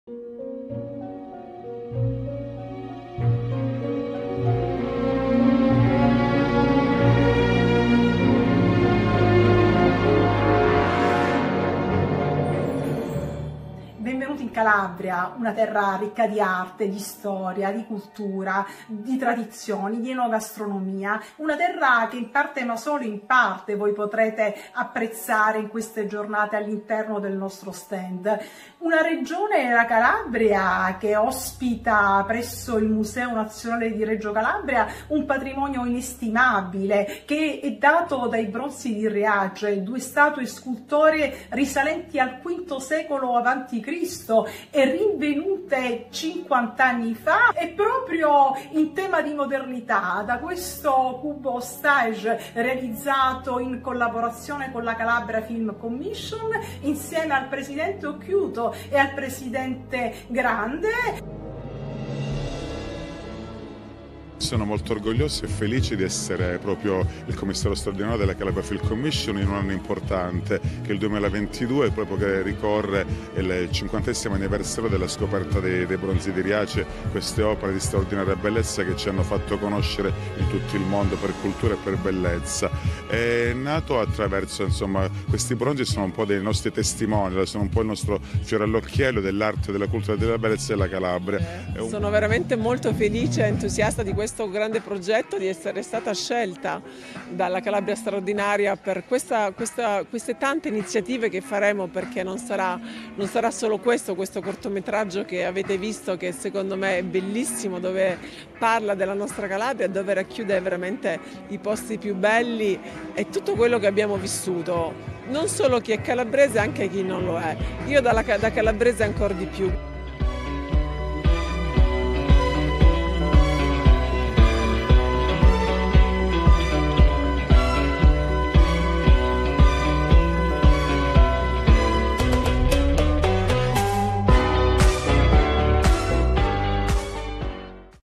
Con i Bronzi di Riace la Calabria alla Festa del Cinema di Roma sul Cube Stage
Eva Giovannini, Nicola Gratteri, Saverio La Ruina, il MIAI – Museo Interattivo di Arte Informatica di Cosenza. Sono solo alcuni degli ospiti del Cube Stage, che torna nel weekend di chiusura della Festa del Cinema. Show immersivi che aumentano la realtà delle performance live di artisti calabresi contemporanei tra i più apprezzati e innovativi. Con gli echi classici della Magna Grecia e con i Bronzi di Riace, meta-celebrity della Festa del Cinema di Roma grazie al virtual-documentary proiettato all’interno del Cube Stage. Appuntamento alla Casa del Cinema, Villa Borghese – Roma nell’ambito delle celebrazioni Bronzi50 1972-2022. Per i prossimi appuntamenti dal 20 al 22 ottobre sono attesi il procuratore di Catanzaro Nicola Gratteri, l’attore Saverio La Ruina, la performer di danza/teatro Maria Luigia Gioffrè. E ancora i musicisti-compositori Remo De Vico, Dario Della Rossa, Massimo Palermo, Alessandro Rizzo del MIAI.

Foto: Luca Perazzolo via HF4
Video: Via HF4

LEGGI ANCHE : Bronzi di Riace, 50 anni dal ritrovamento: l’evento di presentazione delle celebrazioni